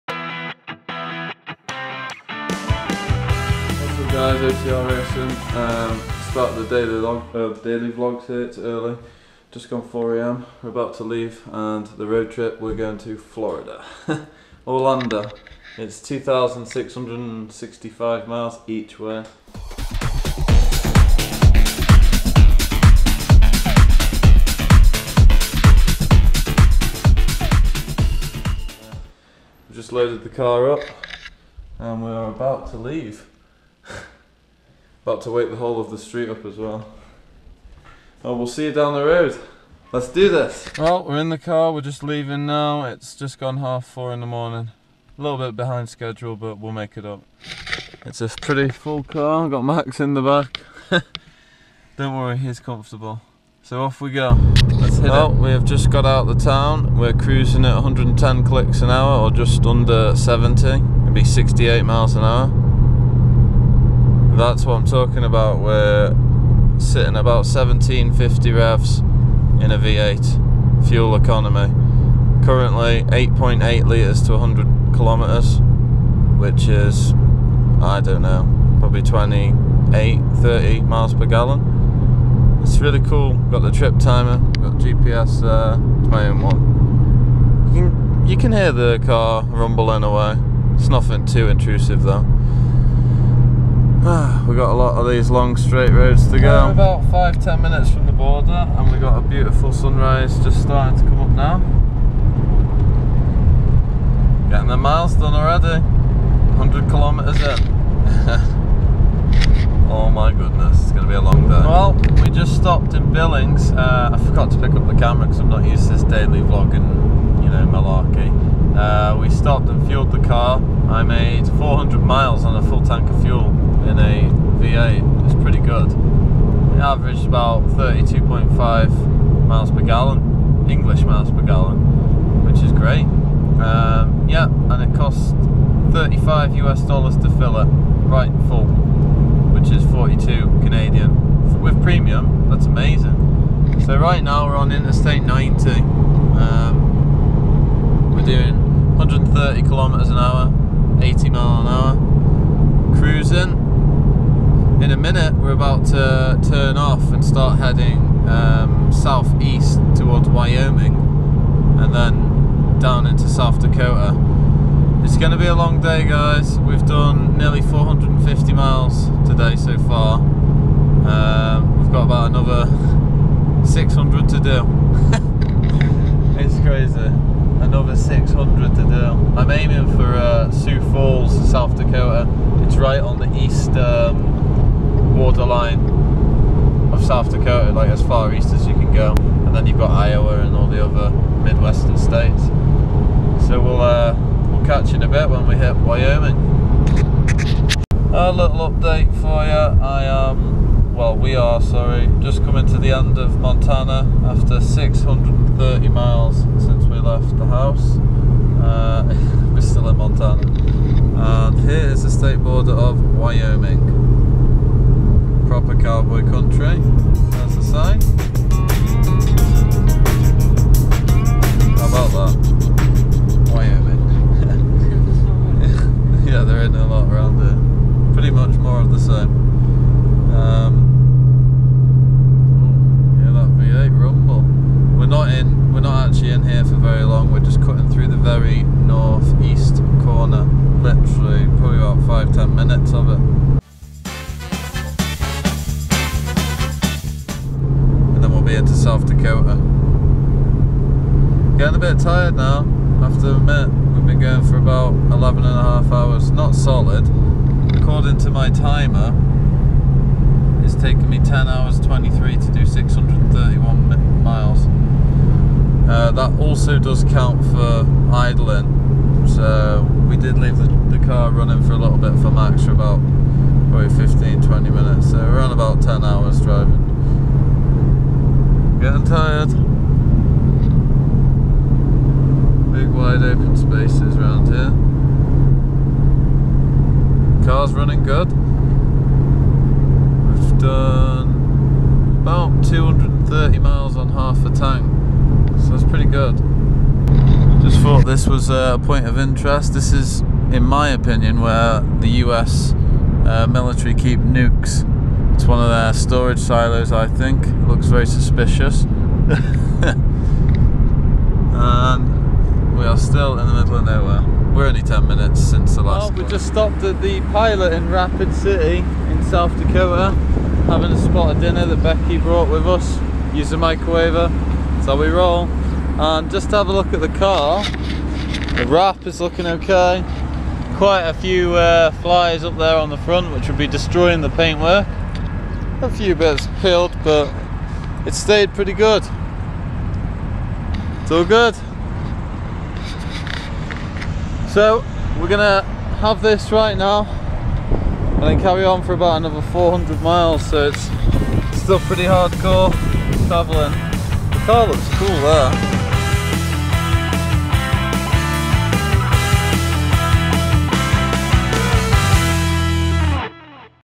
Road Trip Across America In A Mustang GT - Day 1
Come Join me on a fantastic Road Trip across America in a Muscle car a 2015 Mustang GT from Lethbridge, Alberta to Orlando, Florida 2660 miles each way, check it out hit subscribe and like and comment